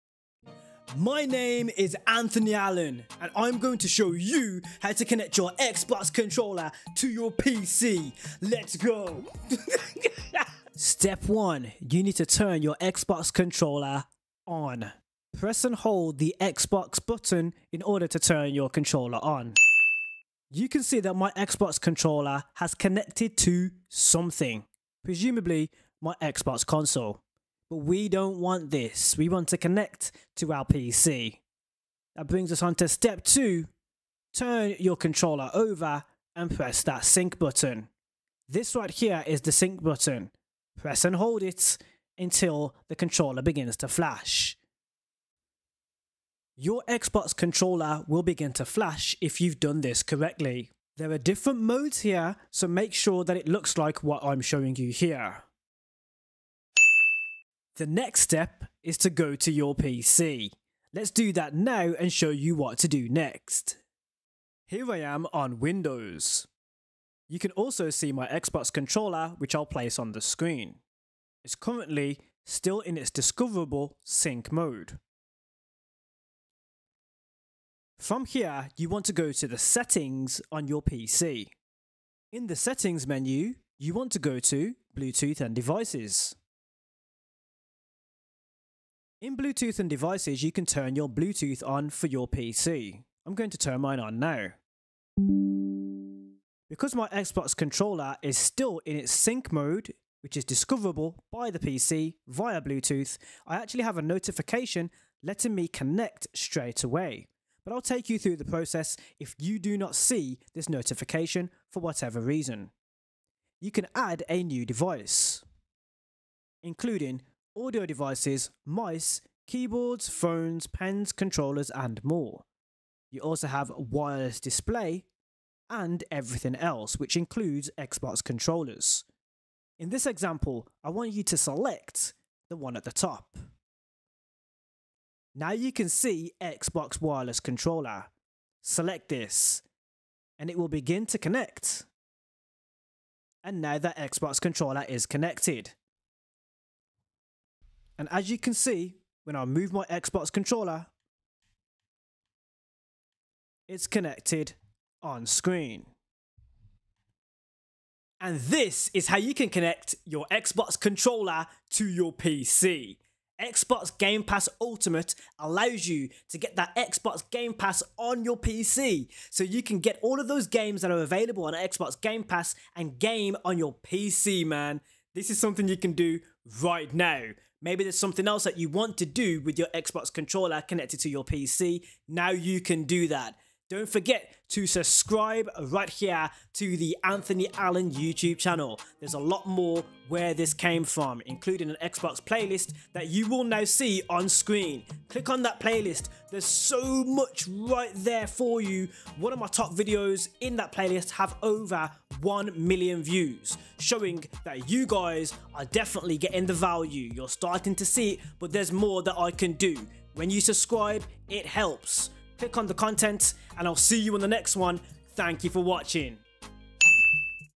How to Connect Xbox Controller to PC For Beginners
How to Connect Xbox Controller to PC For Beginners Want to connect your Xbox controller to your PC? In this video shows how to connect xbox controller to pc,xbox controller,xbox controller pc using your xbox controller and microsoft windows pc watch these easy steps for beginners on how to connect your Xbox controller to your PC with ease!